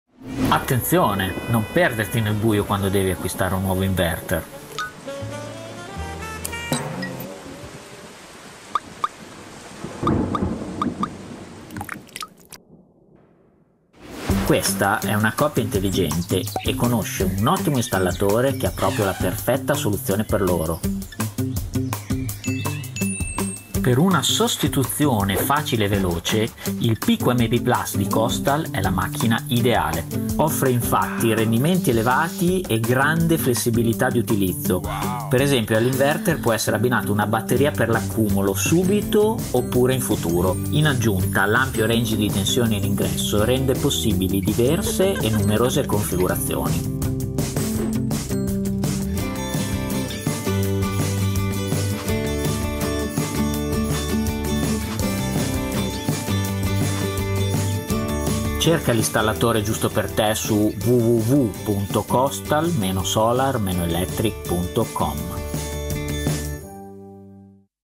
InstallerSmart: PIKO MP plus - rinnovamento con KOSTAL
KOSTAL diventa un cartone animato: questa breve e leggera animazione mostra quale sia la miglior soluzione quando si deve sostituire un vecchio inverter in un impianto solare residenziale, raggiungendo così migliori prestazioni e possibili nuove integrazioni.

Sulla trama:
Quando il vecchio inverter passa a miglio vita e deve essere sostituito, molti utenti si fanno prendere dallo sconforto.
Irritati, cercano una soluzione rapida ed efficiente.
Non è così per la famiglia Smart che affronta il problema in modo intelligente: conosce un bravo installatore, Leo Lightning, il quale trova rapidamente il giusto inverter per il loro piccolo impianto residenziale: L’inverter monofase ibrido della KOSTAL, il PIKO MP Plus. La famiglia Smart è stupita di ciò che è capace di fare il piccolo inverter…

Riguardo il dispositivo:
Il PIKO MP Plus è l’inverter monofase della KOSTAL per impianti residenziali e piccolo-commerciali. Si adatta ad ogni esigenza, sia per le nuove installazioni ma anche per il retrofit. Può essere utilizzato sia come inverter di stringa puro, sia come inverter ibrido (solare e batteria su una singola macchina) e infine anche come inverter solo per la carica/scarica di una batteria.
Vantaggi e caratteristiche del prodotto:
Connessione AC: monofase
Uscita AC: 1.5 – 5 kW
1-2 inseguitori MPP
Connessione batteria semplice
Certificazione IP 65 per utilizzo interno ed esterno
Garanzia KOSTAL gratuita
Monitoraggio in tempo reale via App e sul Solar Portal
Compatibile con wallbox
Possibile integrazione di ulteriori fonti di energia
Possibile connessione e controllo di carichi energivori in modalità Smart Home

Let’s connect!